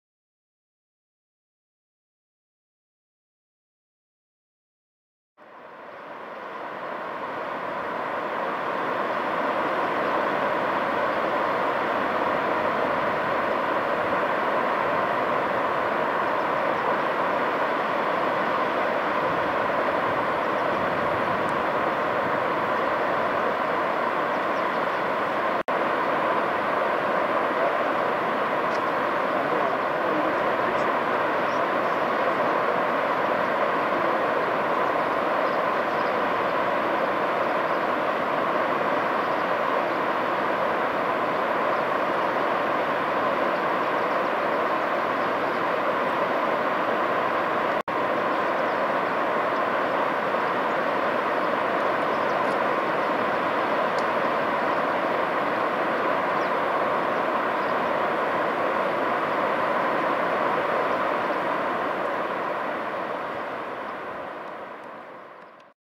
Calcite Springs (HD)
Calcite Springs

Hot springs are sites where groundwater emerges at the Earth’s surface (or on the seafloor).  Hot spring water has to be higher in temperature than the human body (an admittedly arbitrary definition): over 98° Fahrenheit or over 37° Celsius.  Geysers are hot springs that episodically erupt columns of water. The highest concentration of geysers and hot springs anywhere is in and around the Yellowstone Hotspot Volcano (northwestern Wyoming, USA).

Calcite Springs is a cluster of hot springs and fumaroles emerging from hydrothermally altered, mixed volcanogenic siliciclastics of the Sepulcher Formation (Eocene) along the steep riverbanks of the northern Yellowstone Canyon, near Tower Falls, in the northeastern part of Yellowstone Park.  These hot springs erupt acidic to alkaline, hot to near-boiling water, plus petroleum (oil and gas condensates).  Hydrothermal and gaseous precipitates from Calcite Springs discharge waters include opal, calcite, gypsum, barite, native sulfur, pyrite, and an undescribed black iron-bearing mineral.  Black-colored water along the edge of the Yellowstone River at Calcite Springs contains abundant fine-grained pyrite (iron sulfide), which is brassy gold-colored in non-powdered form.  Identified extremophile microbes recovered from Calcite Springs runoff channels include one species of Aquificales bacteria, one species of Korarchaeota archaea, and three species of Crenarchaeota archaea.

Clips 1-3 - Calcite Springs in the mid-afternoon of 2 June 2014.